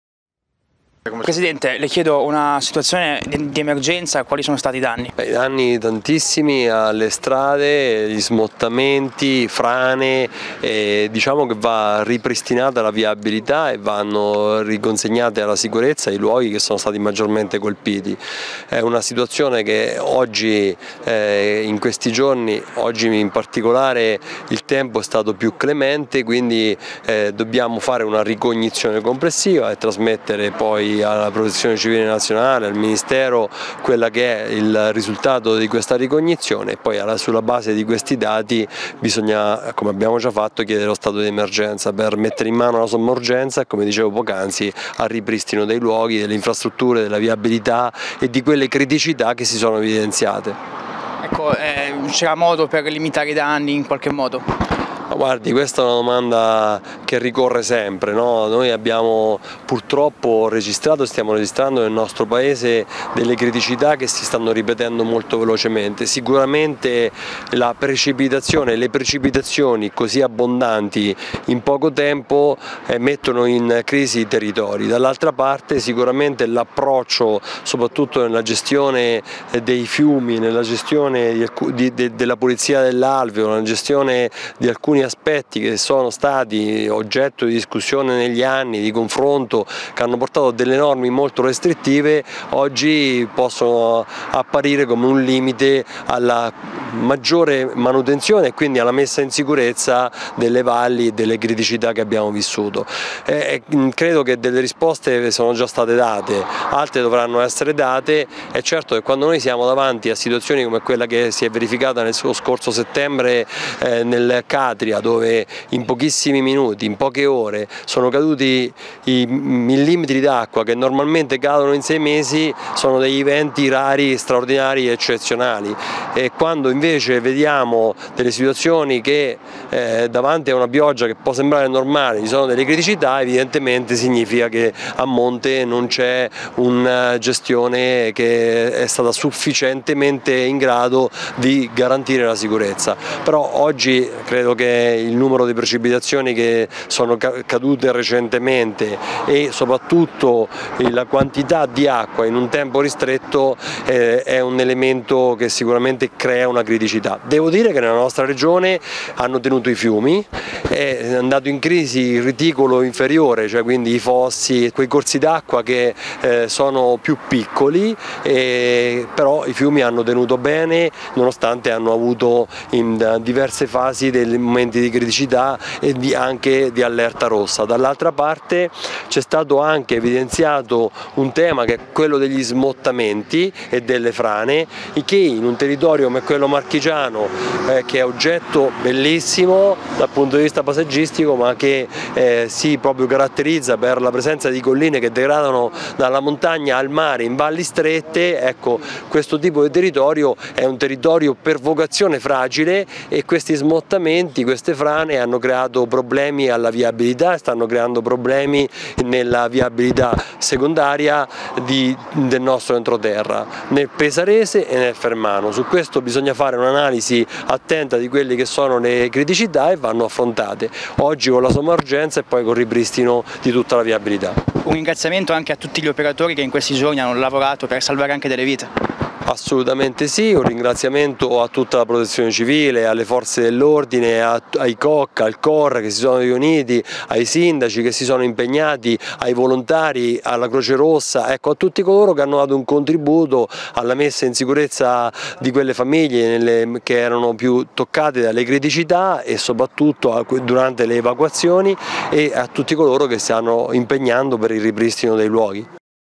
Acquaroli a Pesaro: "Necessario ripristinare i danni"
Martedì il Consiglio dei Ministri sull'emergenza maltempo. Il 19 maggio sopralluogo del governatore Francesco Acquaroli nelle zone della provincia pesarese più colpite dall'alluvione. Tutti al lavoro per ripristinare i danni e la viabilità.